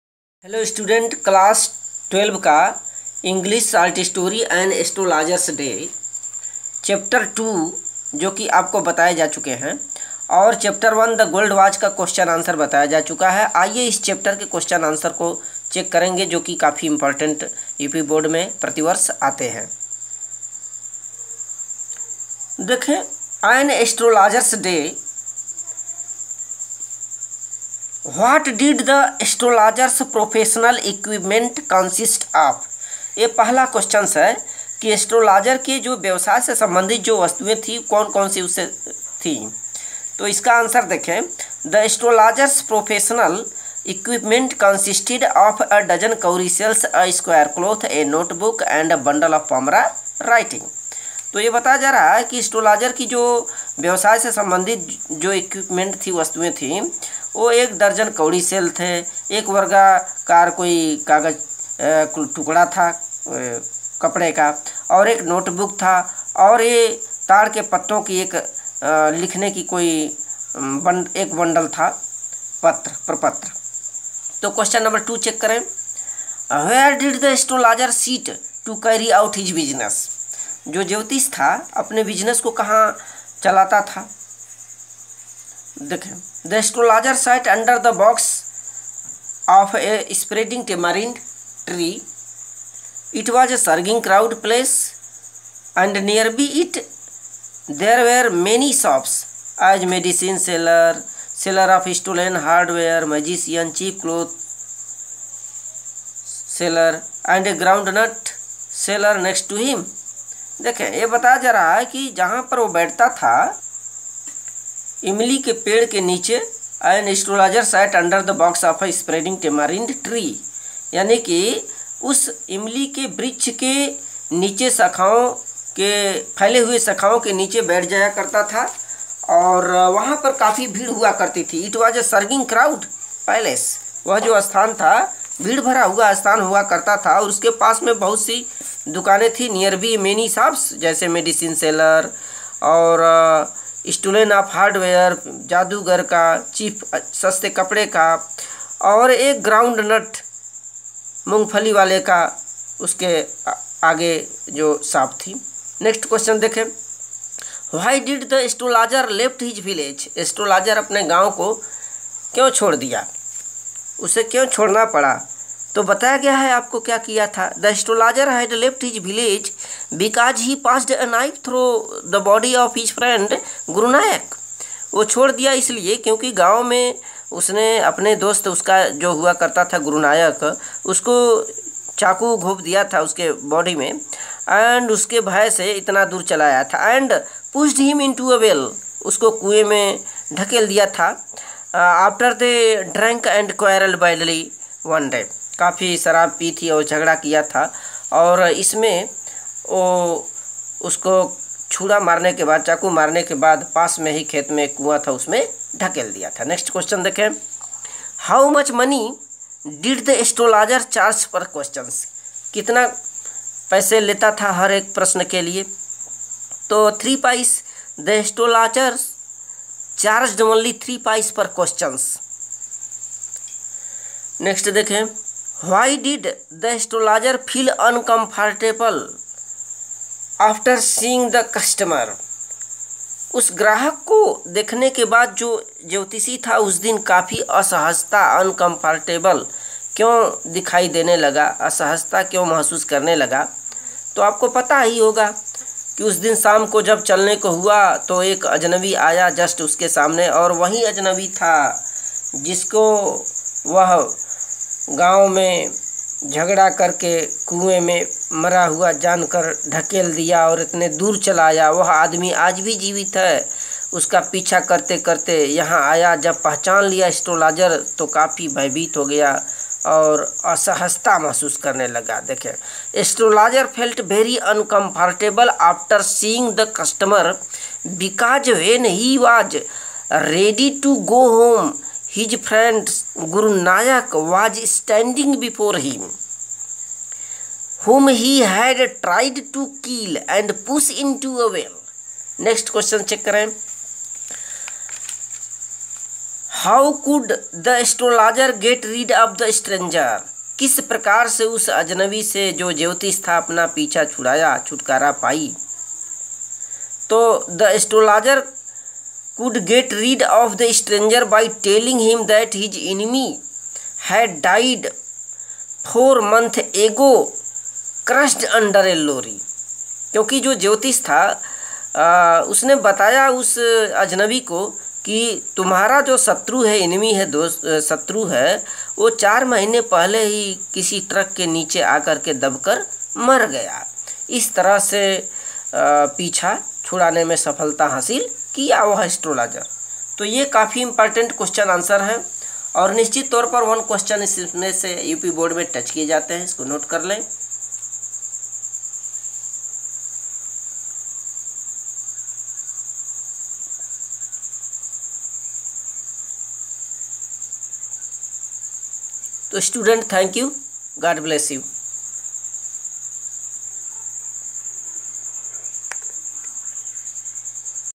An Astrologer's Day by R.K.Narayan in Hindi. Important questions and answer
An Astrologer's Day by R.K.Narayan in Hindi. Up board important questions and answers.